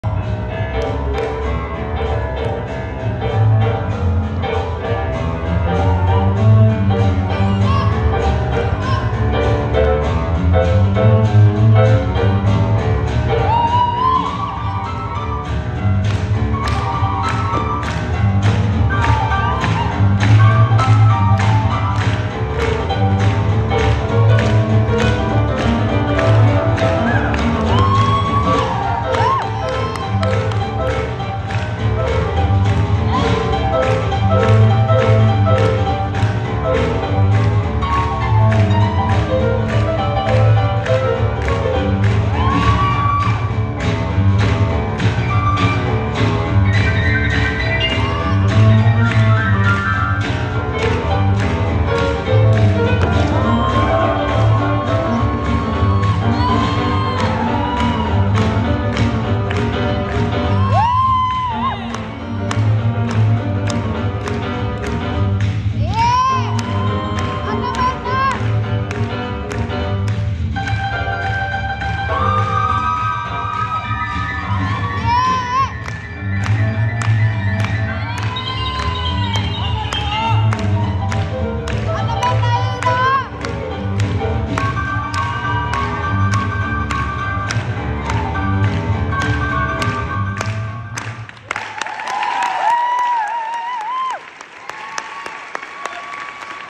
Juho Päivinen & Ida Keränen - Boogie Woogie Fast Final - Finnish Championship 2022
Finnish Championship in Helsinki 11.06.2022

1. Masi Saurén & Anna Tyutyunyuk (Comets, Helsinki)
2. Juho Päivinen & Ida Keränen (Comets, Helsinki)
3. Kimi Manner & Ella Äijänaho (Baunssi, Hämeenlinna)